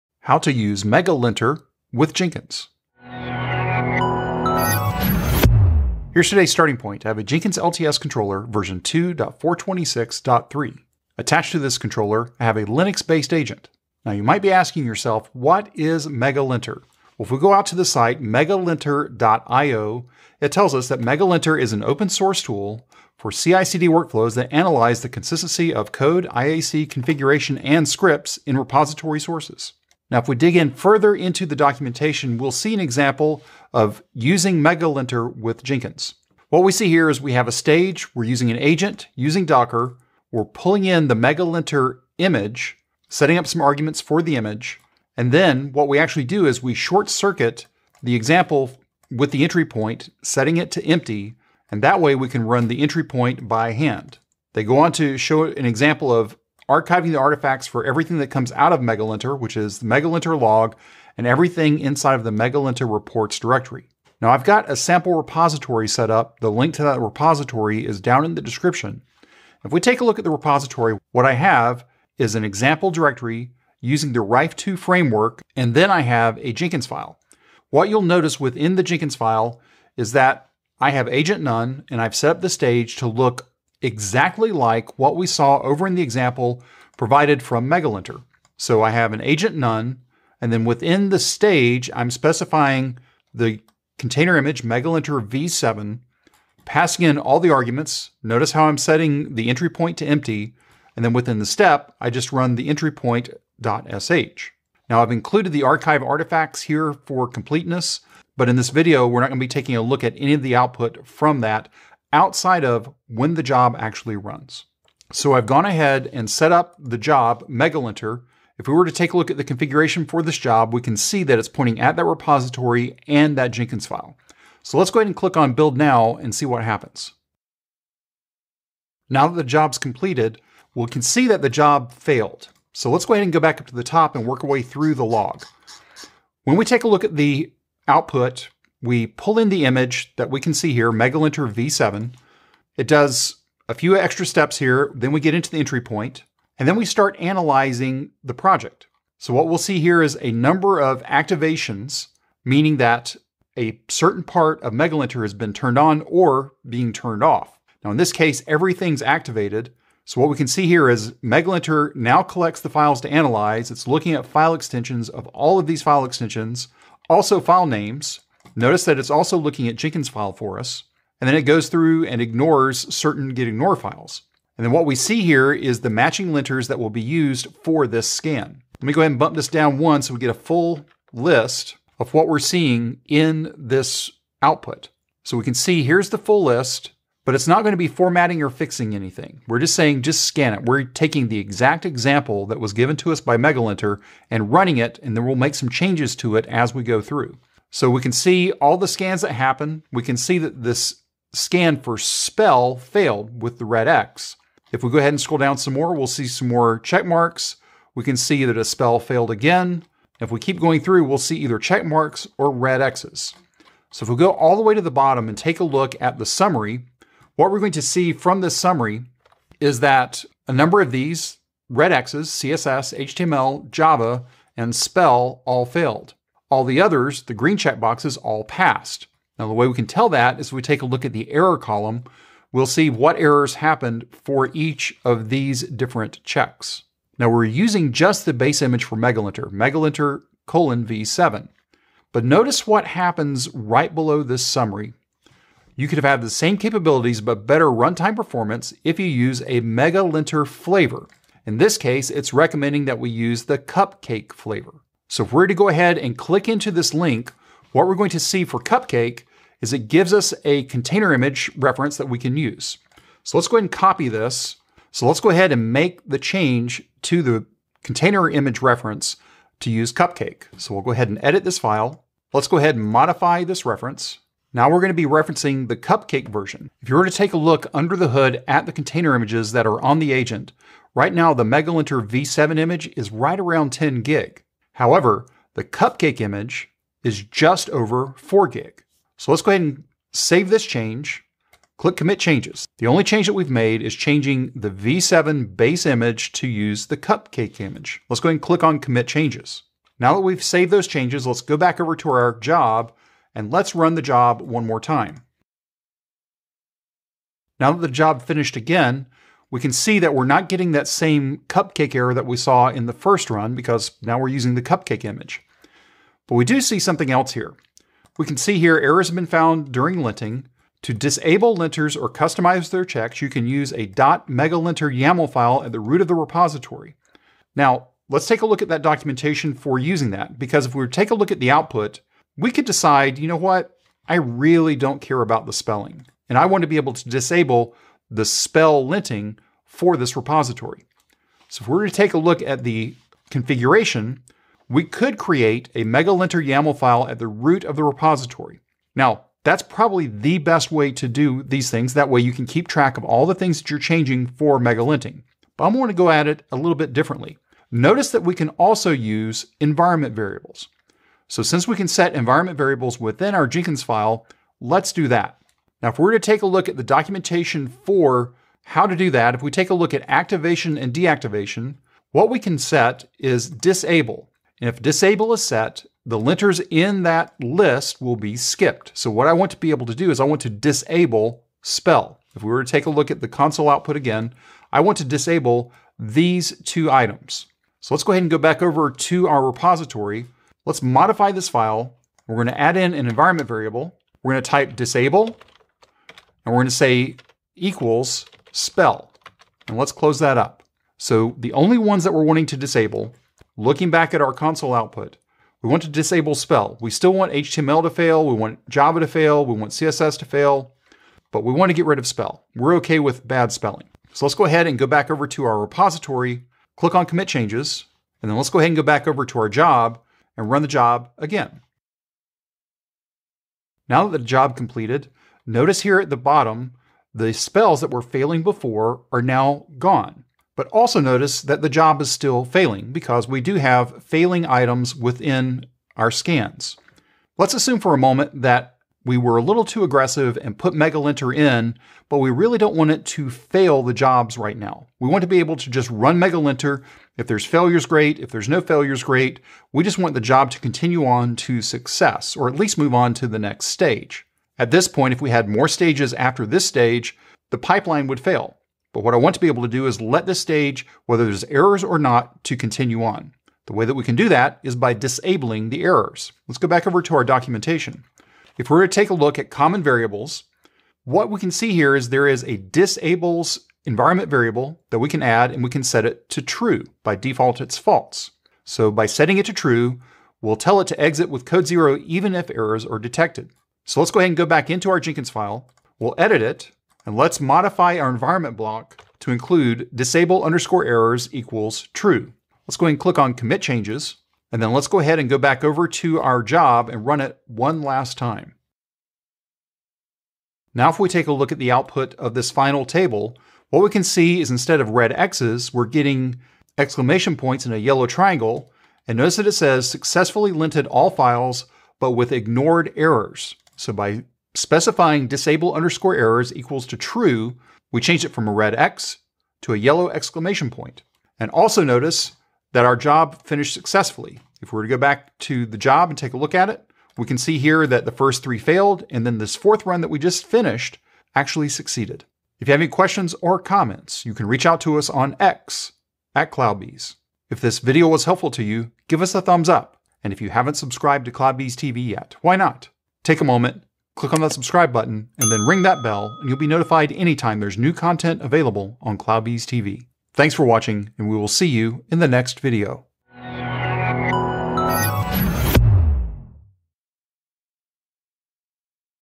How To Use MegaLinter With Jenkins
Timecodes ⏱:

00:00 Introduction
00:05 Review MegaLinter
01:09 Review sample repository
01:54 Run the sample job and review output
04:01 Change the container image used in the Jenkinsfile
05:27 Disable the SHELL linters
07:17 Disable the job from failing when errors are found